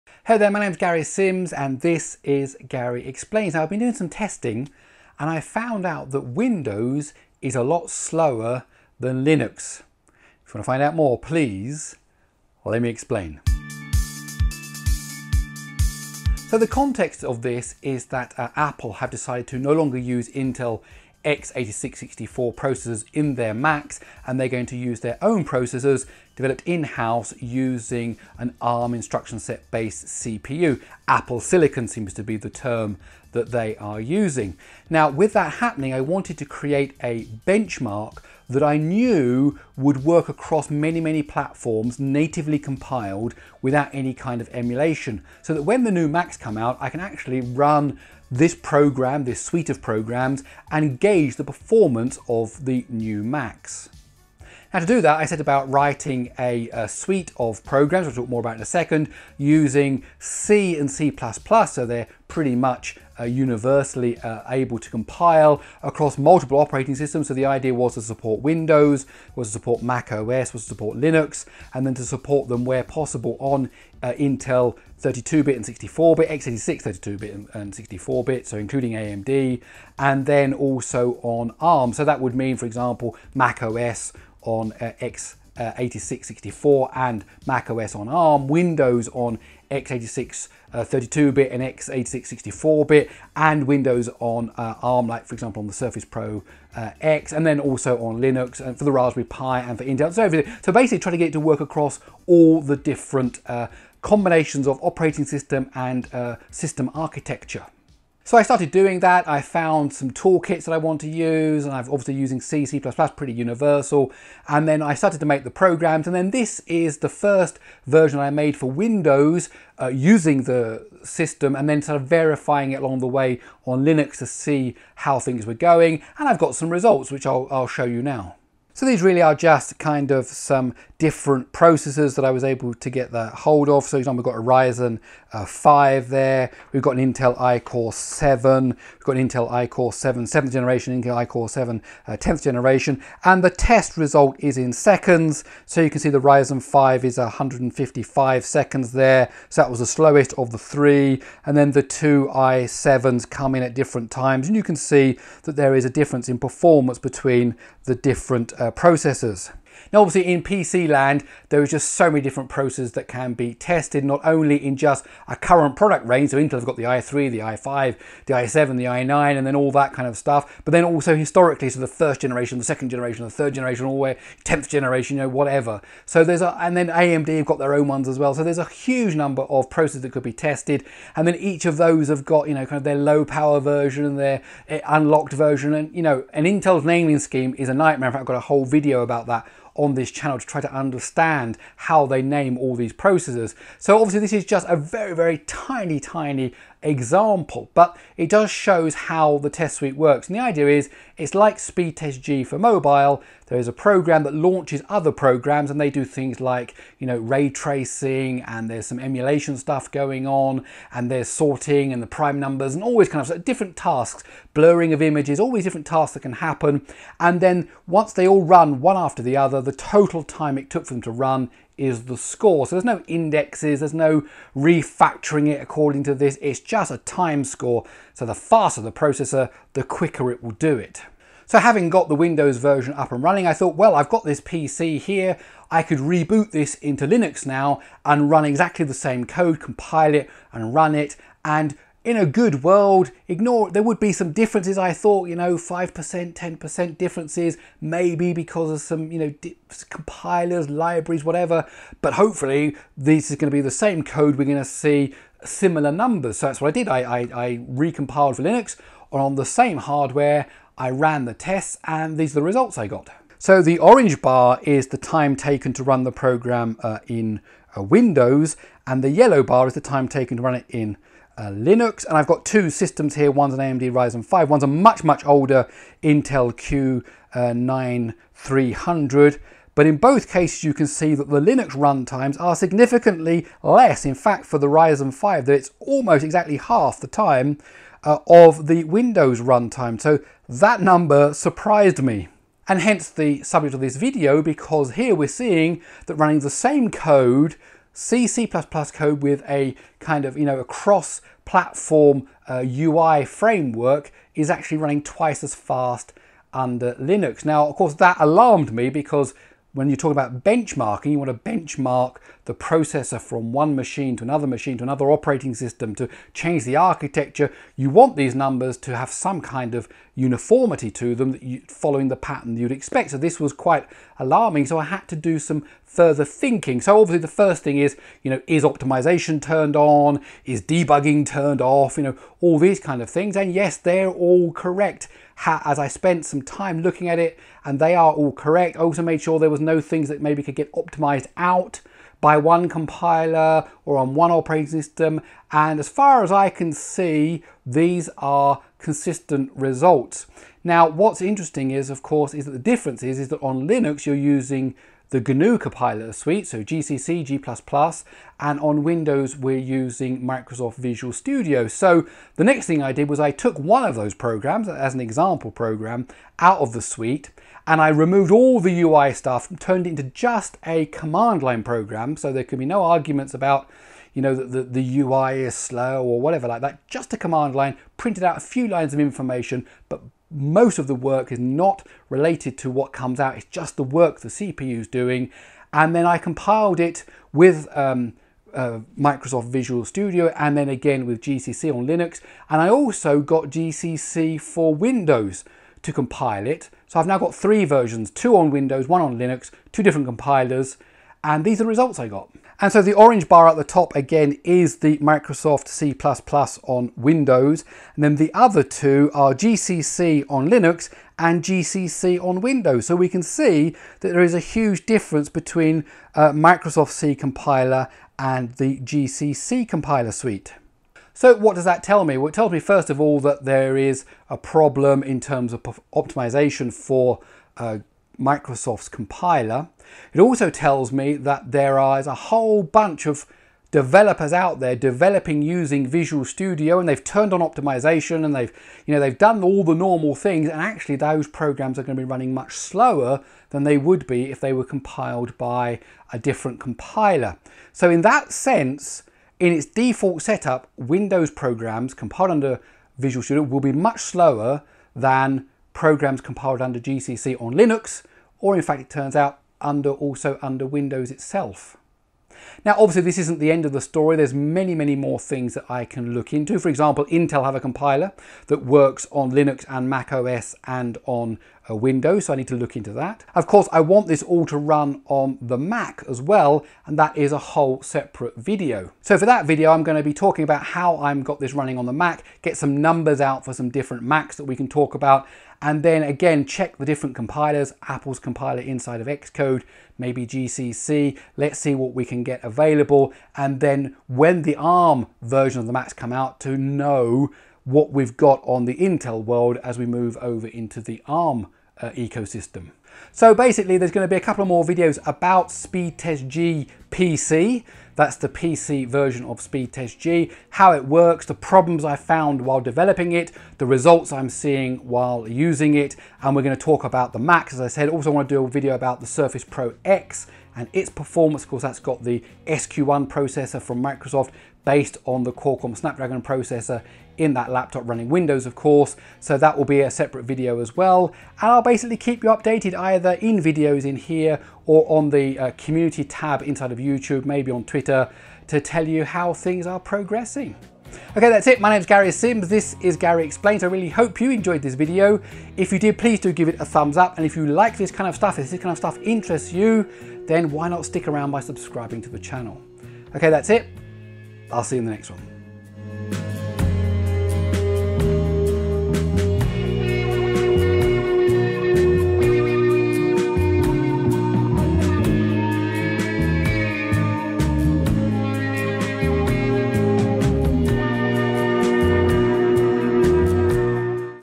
Linux is faster than Windows - Is it the compiler?
While developing my new benchmark, dubbed Speed Test G PC, I noticed that the results were much slower on Windows compared to Linux. Here is why!